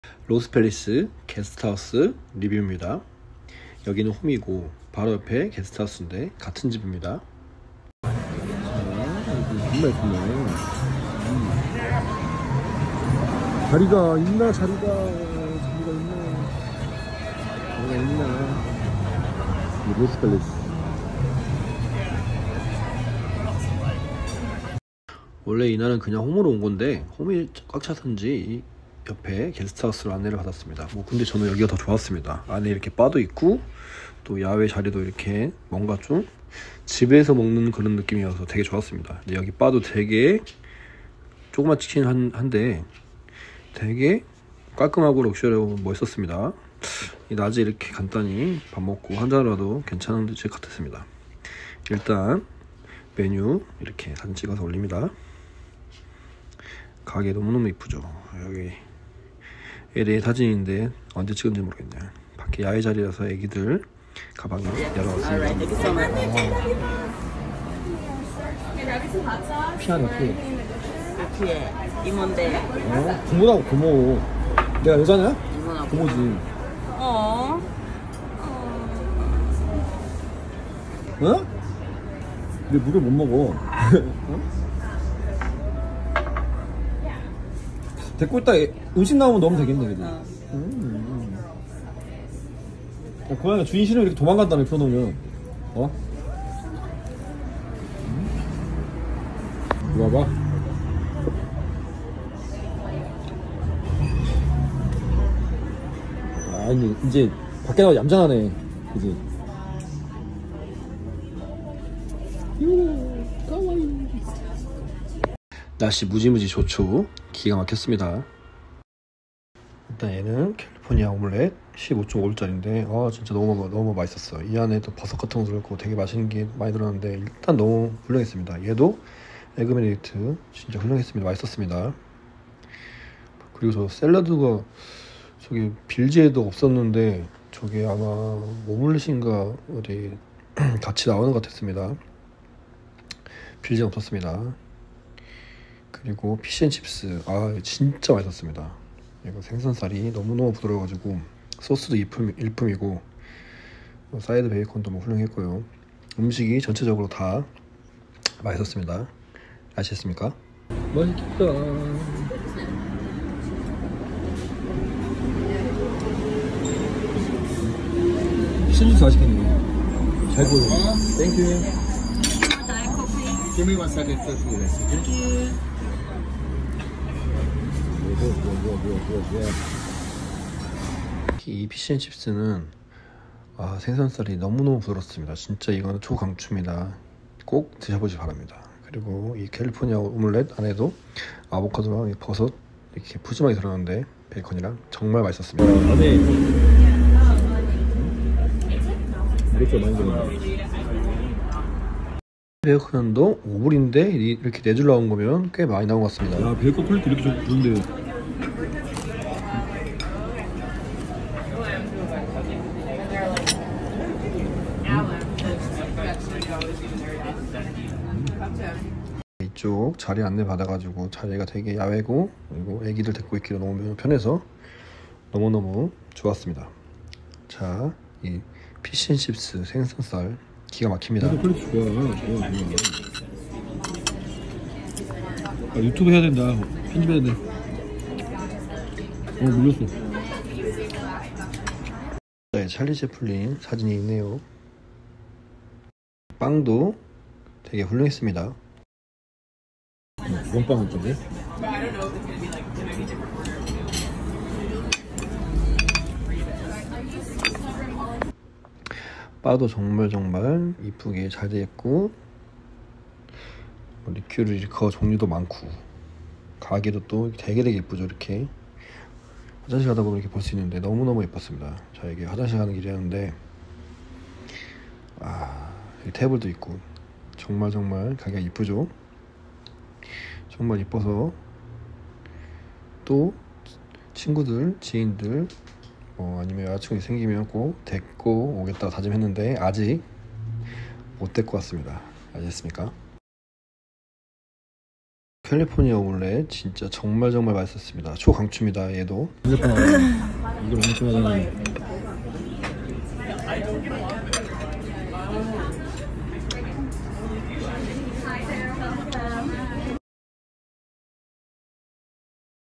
LA 로스 펠리즈 Guest House 먹방 리뷰 feat. 고양이 LA Los Feliz Guest House eating Show Review feat. Cat
고독한 미국미식가 원강이의 슬기로운 미국생활

03/13/2023

1년 1개월 보름전